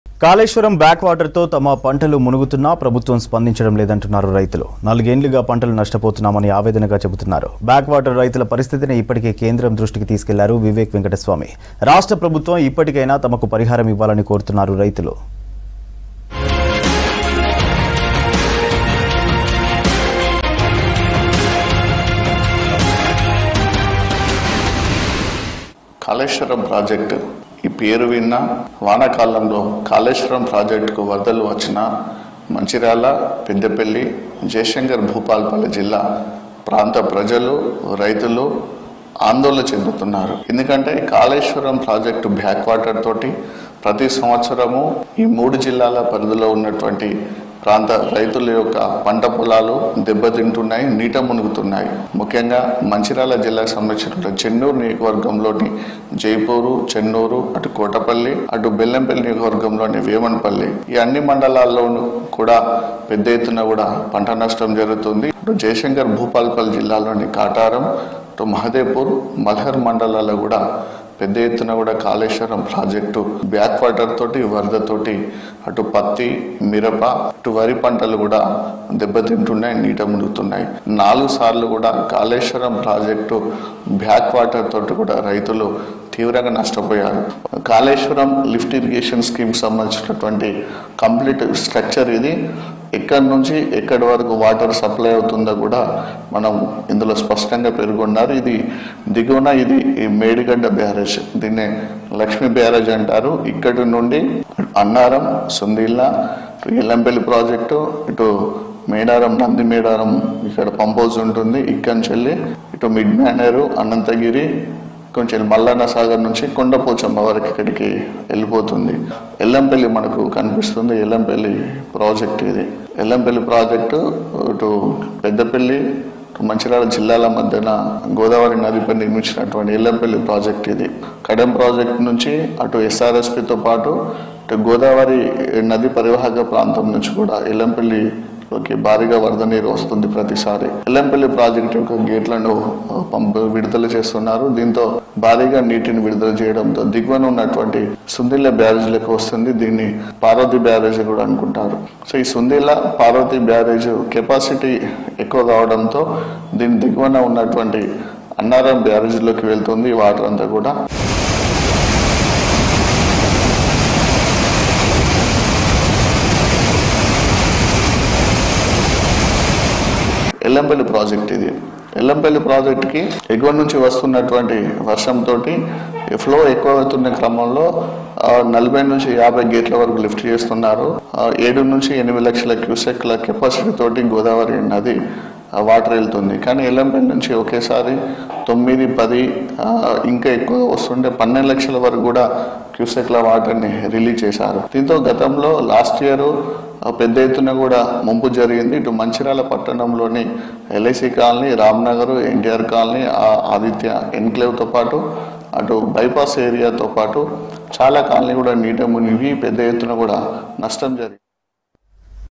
50000 Acres Crop Submerged In Kaleshwaram Project Back Water | V6 News
50000 Acres Crop Submerged In Kaleshwaram Project Back Water  | V6 News